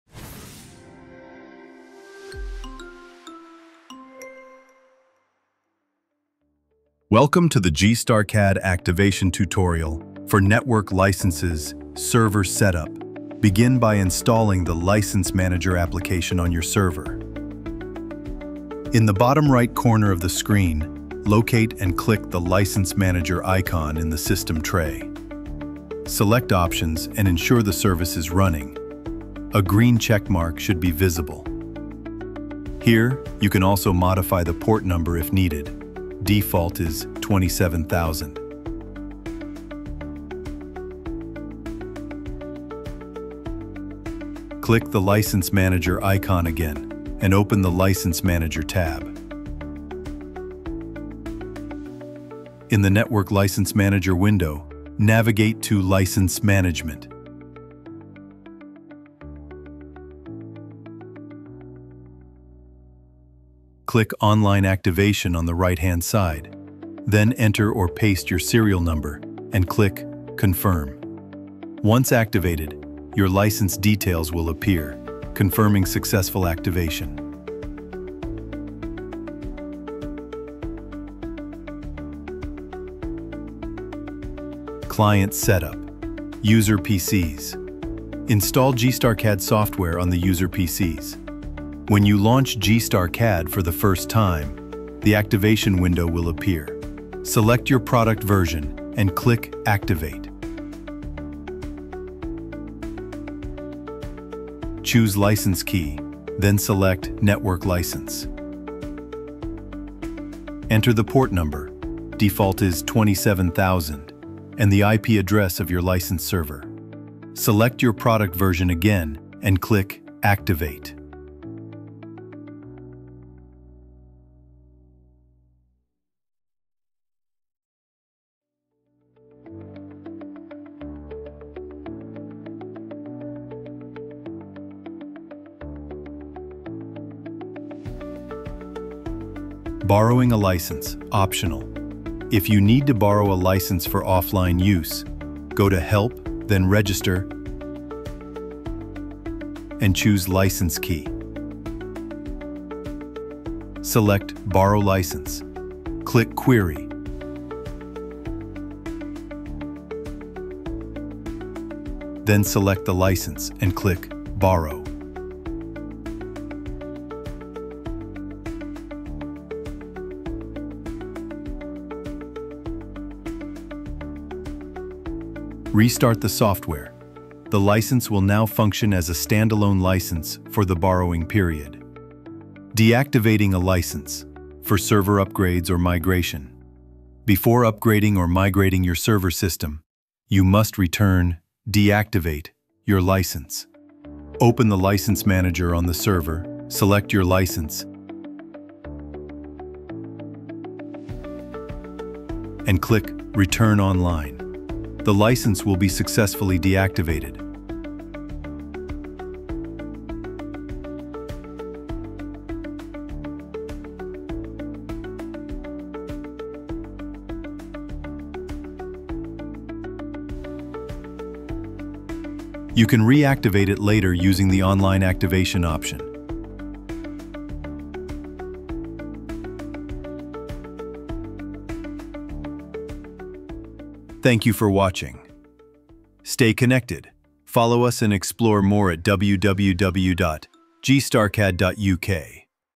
GstarCAD 2026 Installation Guide - Online Activ. for Net-work license (ENG)
Learn how to activate your GstarCAD Network license.